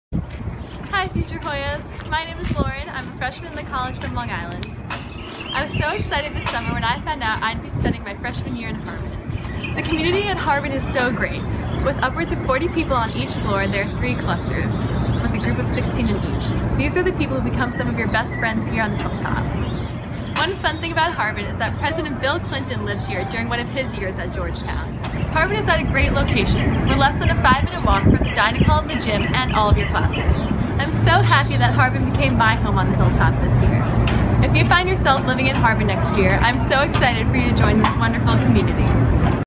Georgetown GAAP Vlog: Harbin
Lauren Quinn (COL '19) talks about one of the four freshman dorms, Harbin!